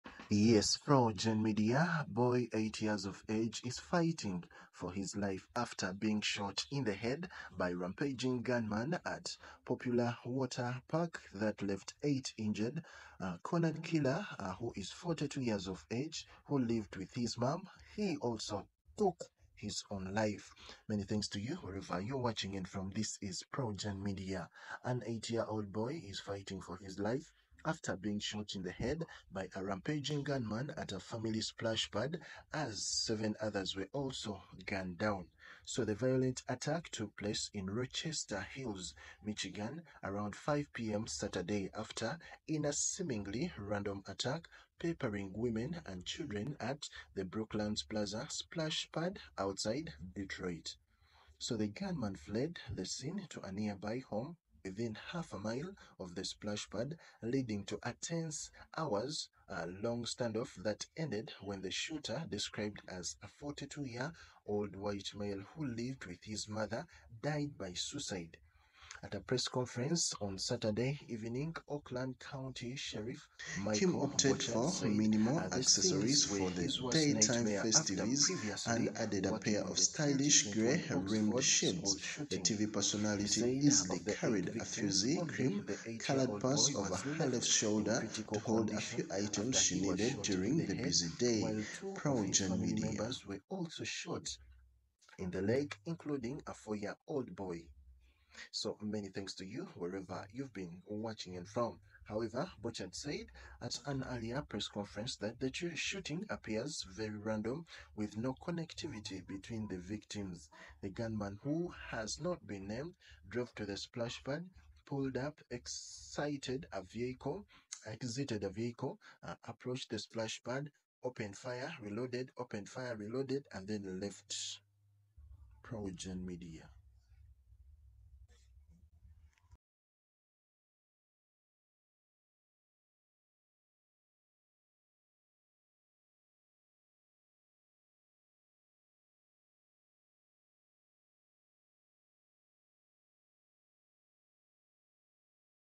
Boy is fighting for his life after being sh*t in the head by rampaging g*nman at popular water park
Boy, 8, is fighting for his life after being shot in the head by rampaging gunman at popular water park that left eight injured: Cornered killer, 42, who lived with his mom takes his own life
An eight-year-old boy is fighting for his life after being shot in the head by a rampaging gunman at a family splash pad as seven others were also gunned down.
The violent attack took place in Rochester Hills, Michigan, around 5pm Saturday after the shooter fired 28 rounds from a handgun in a seemingly random attack, peppering women and children at the Brooklands Plaza Splash Pad outside Detroit.

The gunman fled the scene to a nearby home within half a mile of the splash pad, leading to a tense hours-long standoff that ended when the shooter - described as a 42-year-old white male who lived with his mother - died by suicide.

At a press conference Saturday evening, Oakland County Sheriff Michael Bouchard said the scenes were his 'worst nightmare' after previously working on the 2021 Oxford School Shooting.

He said of the eight victims, only the eight-year-old boy was left in critical condition after he was shot in the head, while two of his family members were also shot in the leg, including a four-year-old boy.